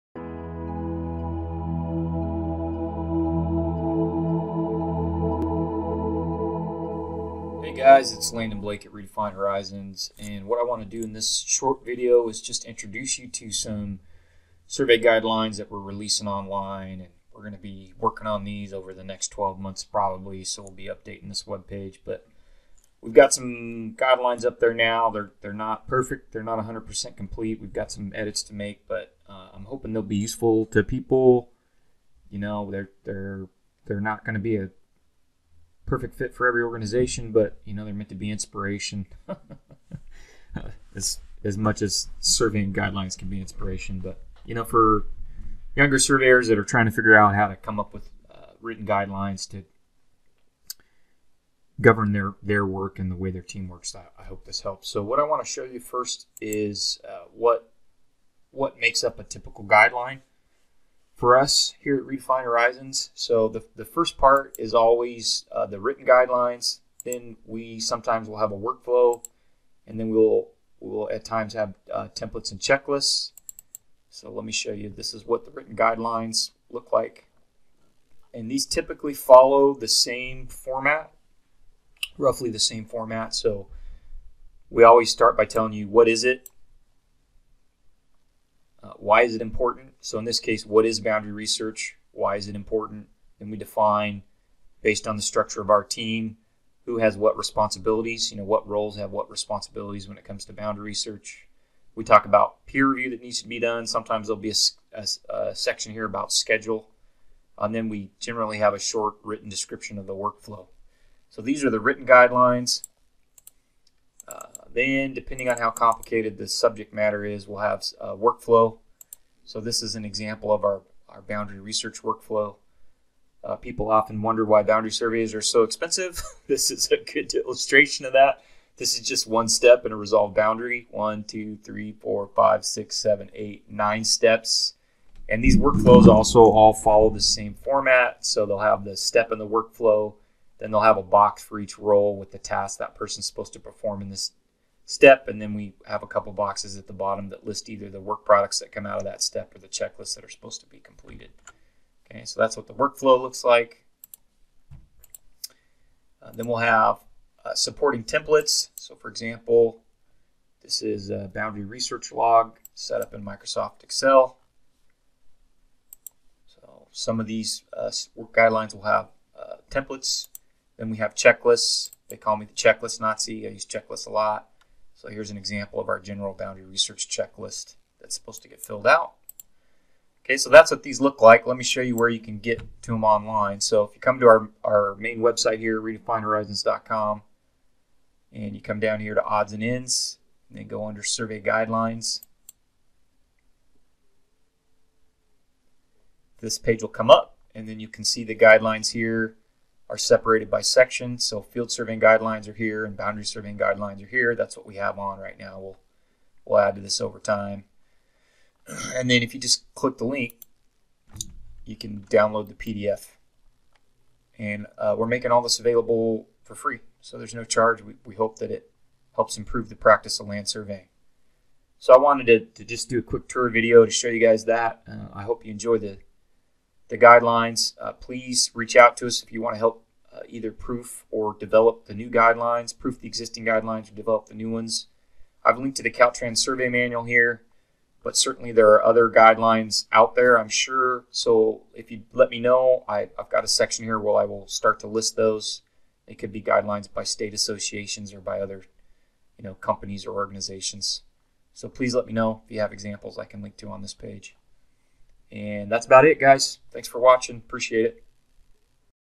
A Quick Overview Of The Redefined Horizons Land Surveying Guidelines
In this video, Landon Blake (the founder of Redefined Horizons) provides a quick overview of the land surveying guidelines Redefined Horizons is making publicly available. In the video he reviews the basic structure of each guideline (and its 4 main parts) and shows you how to download those guidelines from the Redefined Horizons web-site.